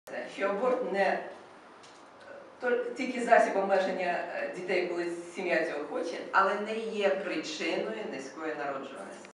1912_13
Освітній семінар-тренінг для представників регіональних та національних ЗМІ на тему «Як професійно висвітлювати питання репродуктивного здоров'я та планування сім'ї, щоб вашу газету та сайт читали, а телеканал – дивилися!» розпочався у Києві. Захід проводить Благодійний Фонд «Здоров'я жінки та планування сім'ї» в рамках реалізації Проекту USAID «Репродуктивне здоров’я в Україні»